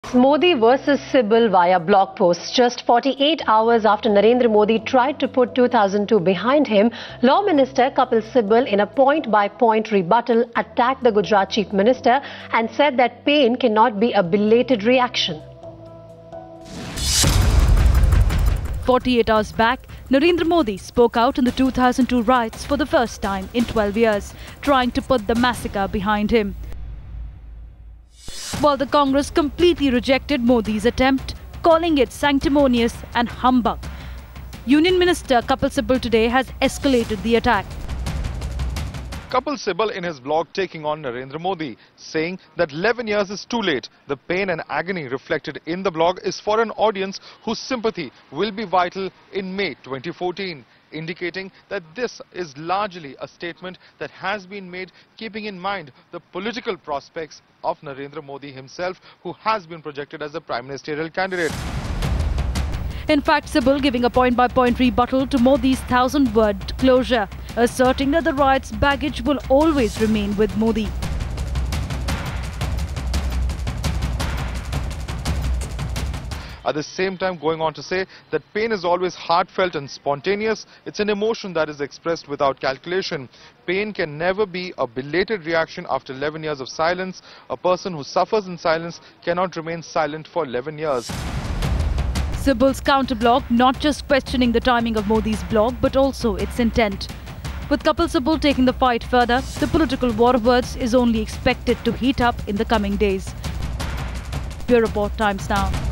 Now, Kapil Sibal takes on Narendra Modi
Union Minister Kapil Sibal on Sunday (Dec 29) claimed that the "pain and agony" reflected in Narendra Modi's feelings over 2002 Gujarat riots was a "belated reaction" aimed at electoral gains during the 2014 Lok Sabha polls and asserted that the "riots baggage" will remain with him. Download the Times Now India's Election HQ app and get all the election info at one go. Social Media Links